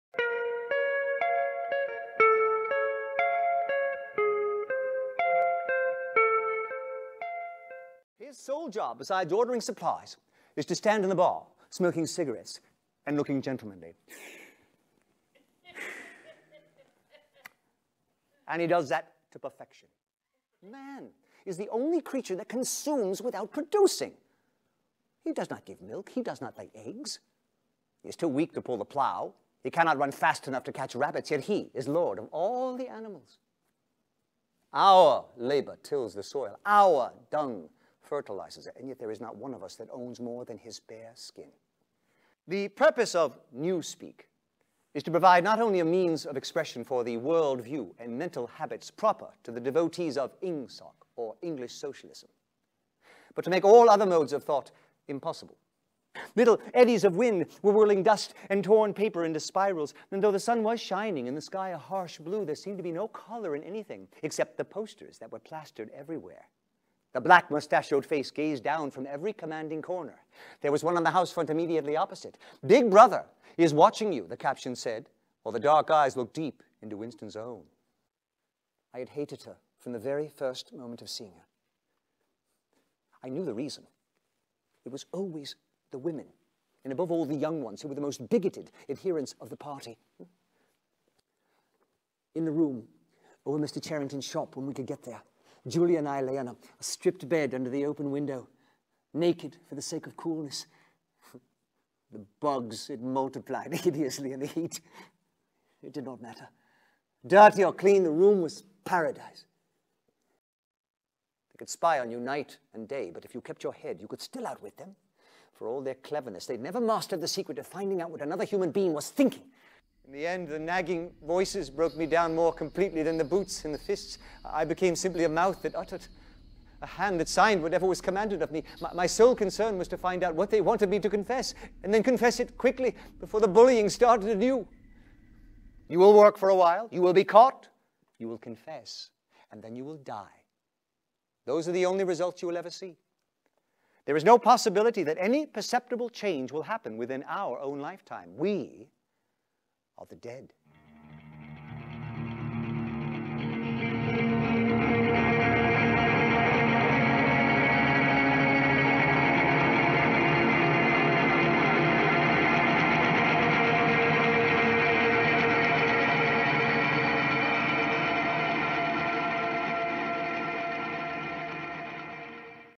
ORWELLIAN, starring Larry Cedar (Preview)
ORWELLIAN, a one-man stage play based on the works of George Orwell, adapted by and starring Larry Cedar. The piece, originally produced by The Porters of Hellsgate Theater Company and directed by artistic director Thomas Bigley, premiered at The Theater of Note as part of  The Hollywood  Fringe Festival. The show later moved to The Odyssey Theatre Ensemble in West Los Angeles, and finally to The Sherry Theater in N. Hollywood as part of the NoHo Fringe Festival where it won Best Solo Performance and was directed by Pamela Cedar.